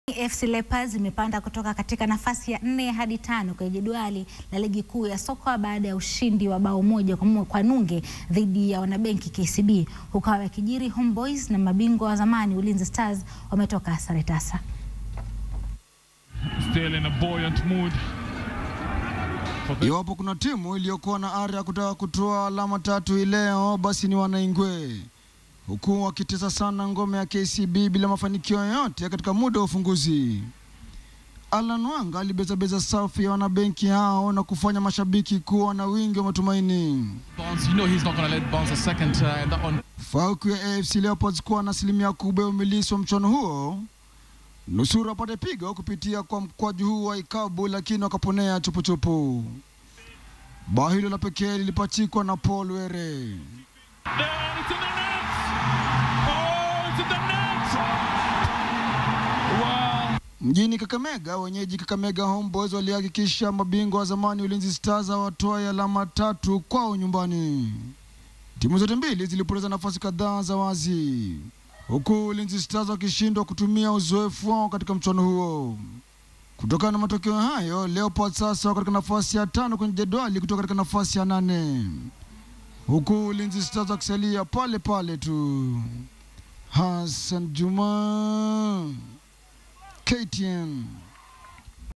AFC Leopards yalaza KCB bao moja bila
AFC Leopards imepanda kutoka katika nafasi ya nane hadi tano kwenye jedwali la ligi kuu ya soka baada ya ushindi wao wa bao moja kwa anunge dhidi ya wanabenki kcb. Huku hayo yakijiri, homeboyz na mabingwa wa zamani ulinzi stars wametoka sare tasa.